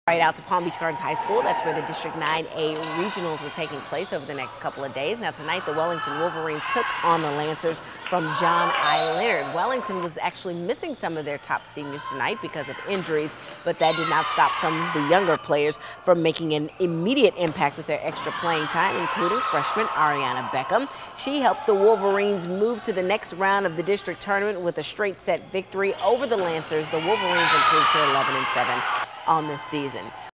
High School Volleyball Playoff Action
John I Leonard and Wellington do battle in girls HIgh School volleyball playoff action